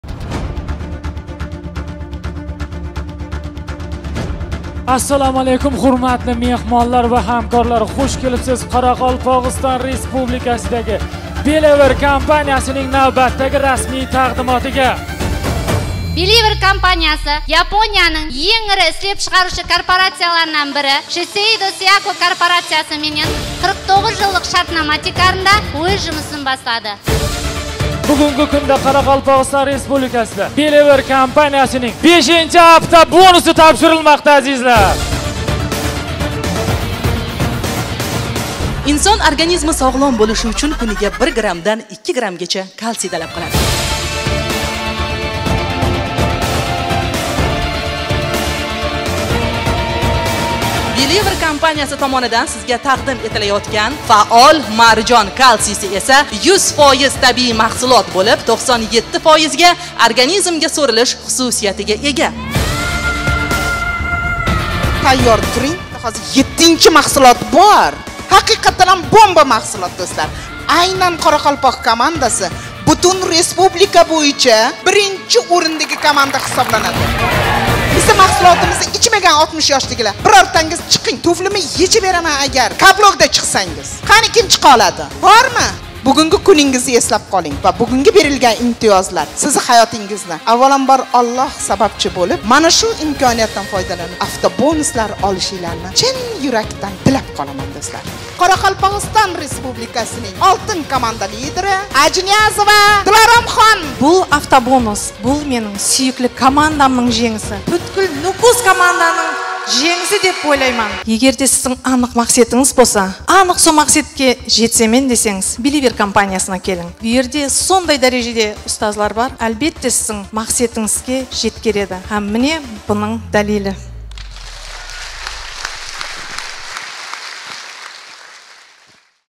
Официальная презентация Каракалпакстан нукус (BELEEVER) 07.08.2022
Официальная презентация Каракалпакстан Нукус: Золотой лидер команды BELEEVER получил свой бонусный автомобиль Cobalt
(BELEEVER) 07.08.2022

ОФИЦИАЛЬНЫЕ СООБЩЕСТВА BELEEVER